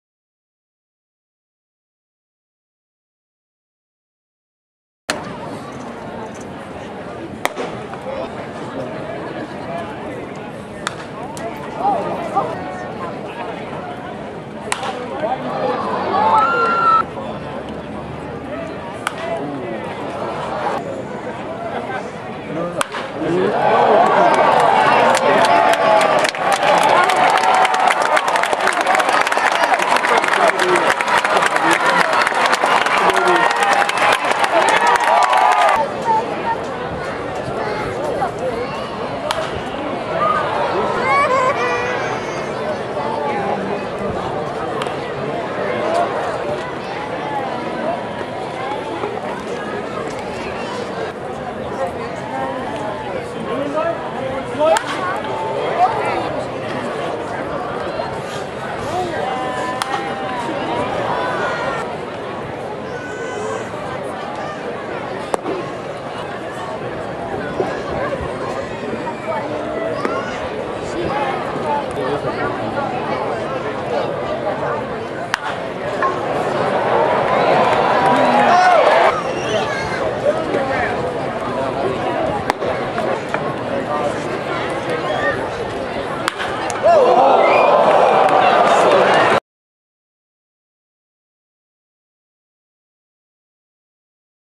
Paul Hendrix - 2B - Mahoning Valley Scrappers (2013-07-05 vs. Williamsport)
Cleveland Indians 2B prospect, Paul Hendrix, vs. Williamsport on July 5th, 2013.